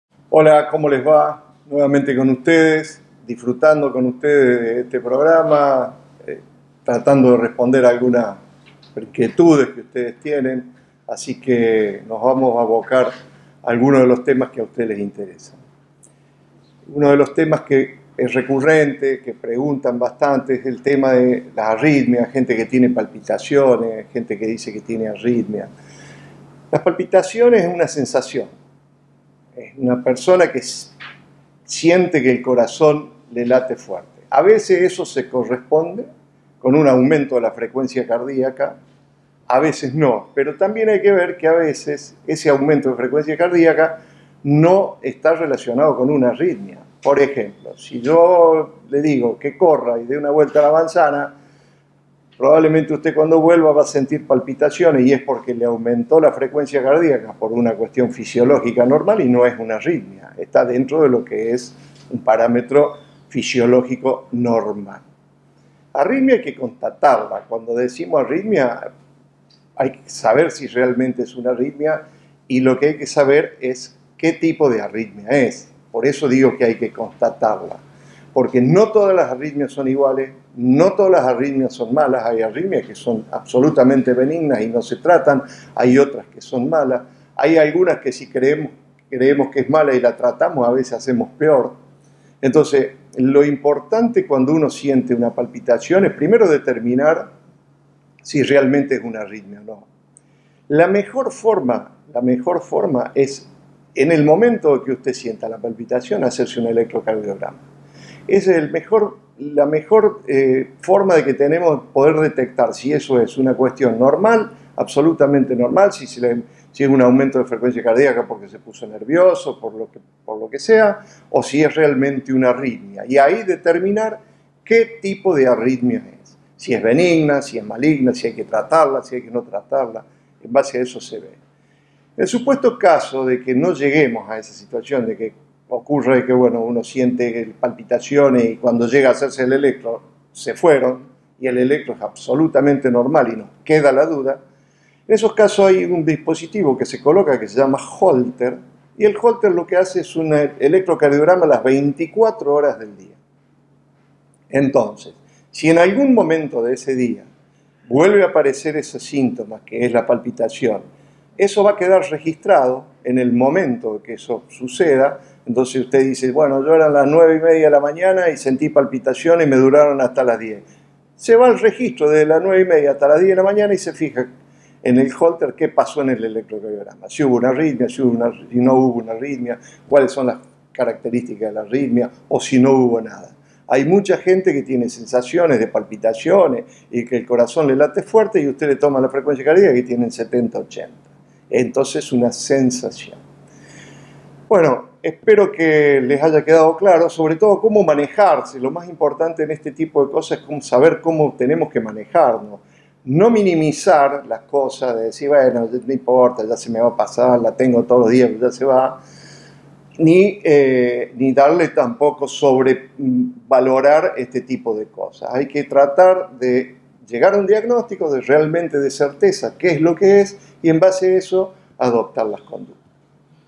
Dr Alberto Licheri "Cardiólogo - Intervencionista" Arritmias y palpitaciones (Parte 1)
El programa de Prevención en Salud, Salud Litoral, tiene la suerte de contar en su staff con el Prestigioso Cardiologo - Intervencionista Dr Alberto Licheri.
A raíz de tantas consultas realizadas por televidentes al teléfono de la producción (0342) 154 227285, fuimos a consultarlo sobre Arritmias y Palpitaciones, sus diferencia y esto nos decía...